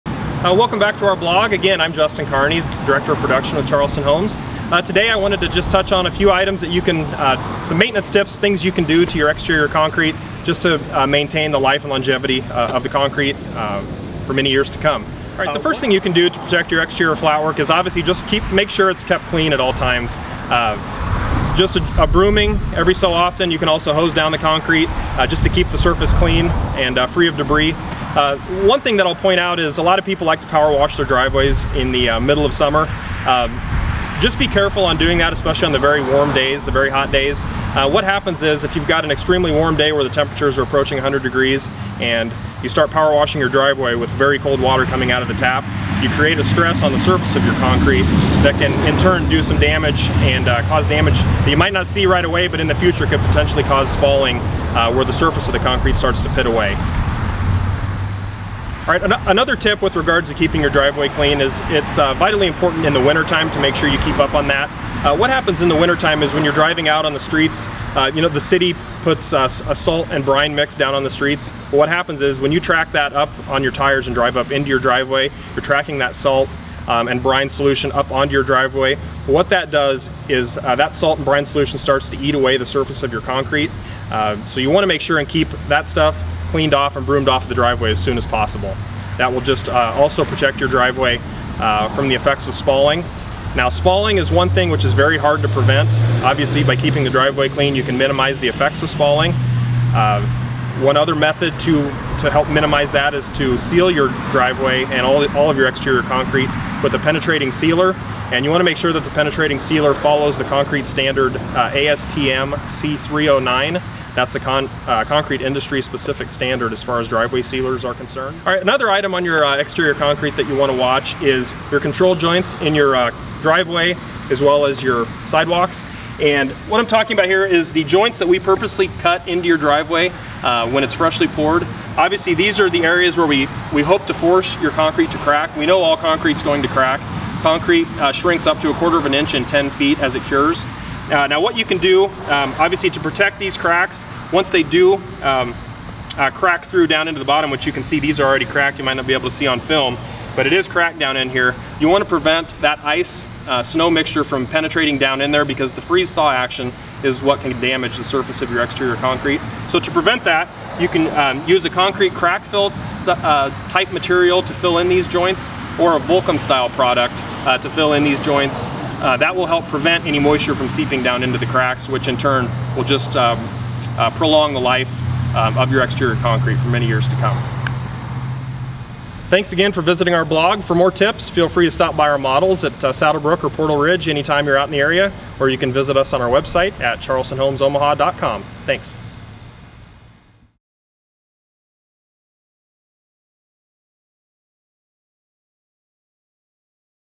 Charleston Homes - Exterior Concrete Part 2
- Did you ever wonder why there are cracks in your driveway? In fact, do you realize that with a few, relatively simple steps, you can treat those cracks and prolong the life of your driveway? For this plus other great tips that will help extend the life of one of the most high traffic areas of your house, go to our video blog and we'll show you how we protect and maintain our driveways.

Charleston Homes
5304 N. 150th Street (Saddlebrook - NW Omaha)
8411 S. 101st Street (Portal Ridge - LaVista)